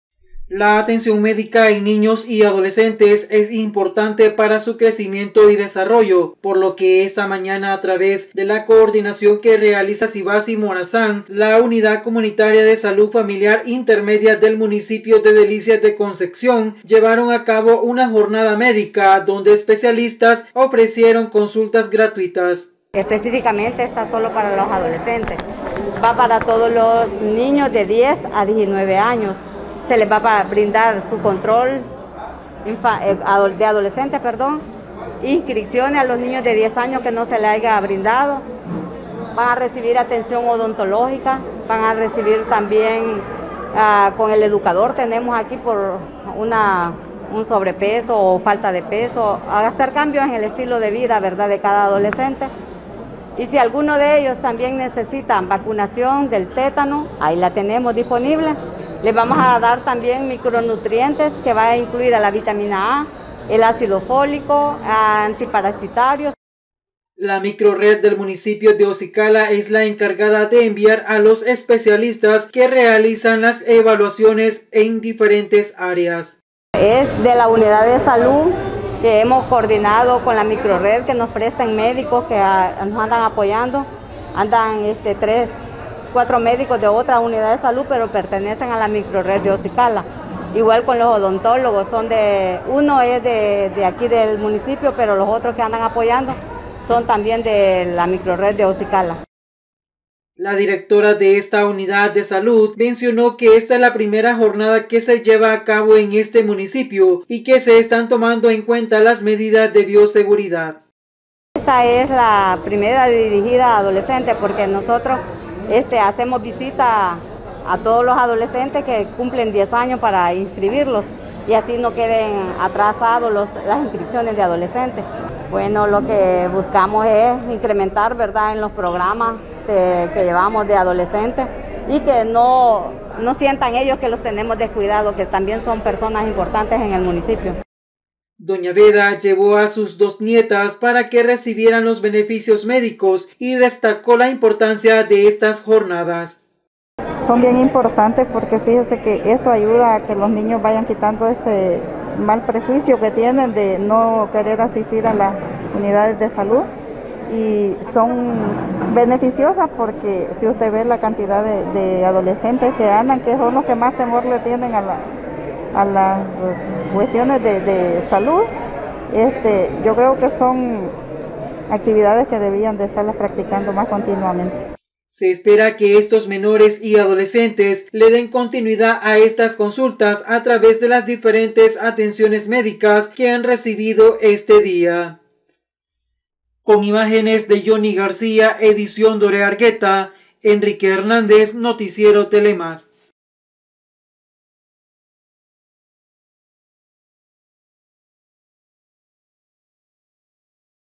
POBLACIÓN DE DELICIAS DE CONCEPCIÓN ES BENEFICIADA CON JORNADA MEDICA
DELICIAS DE CONCEPCION